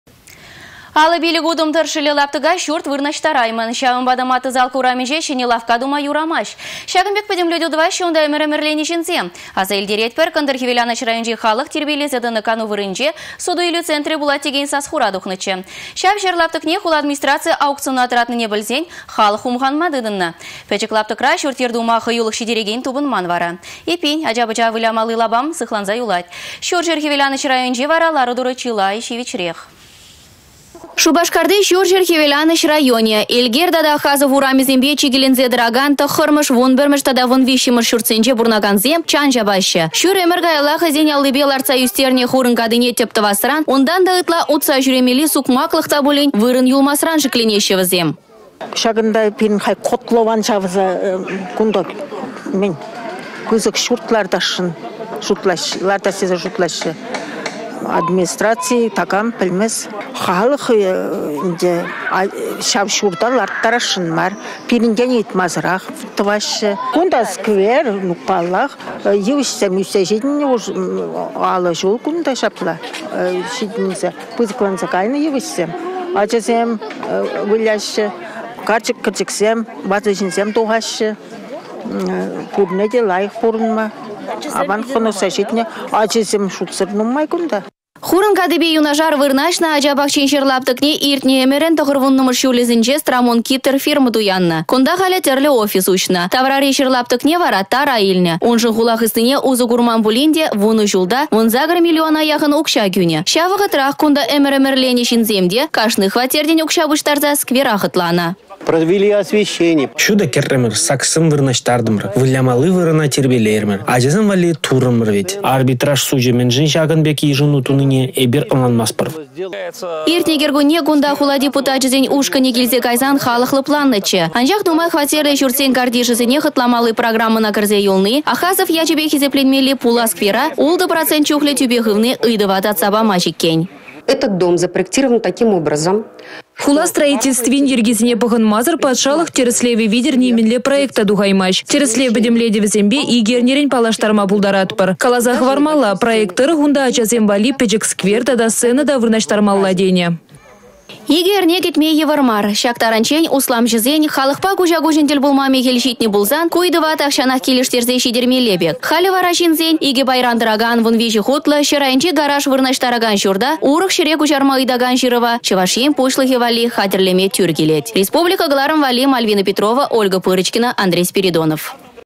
«Шаннă кайăк йăвара мар»
55 утăм тăршшĕллĕ лаптăка çурт вырнаçтараймăн. Çавăнпа та  Мате Залка урамĕнче çĕнĕ лавкка тума юрамасть. Çакăн пек пĕтемлетӳ тăваççĕ унта ĕмĕр ĕмĕрленĕ çынсем.  Аса илтеретпĕр, Кăнтăр-Хĕвеланăç районĕнчи халăх тирпейлесе тăнă кану вырăнĕнче  суту-илӳ центрĕ пулать текен сас-хура тухнăччĕ. Çав çĕр лаптăкне хула администрацийĕ аукциона тăратнине пĕлсен халăх хумханма тытăннă. Пĕчĕк лаптăкра çурт-йĕр тума хăюлăх çитерекен тупăнман. Эппин, ача-пăча вылямалли лапам та сыхланса юлать. Çурçĕр-Хĕвеланăç районĕнче вара  лару-тăру чылай çивĕчрех.